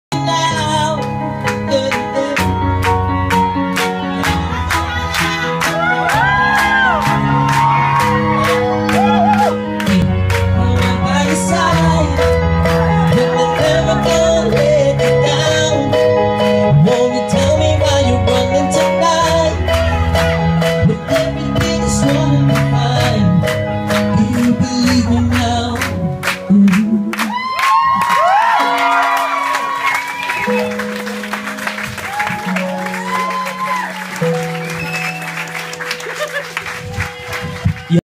Glamboy P singing at Carla & Fionas communion
Gardenparty in ekstrem rain. Carla and Fionas communion in Hellerup.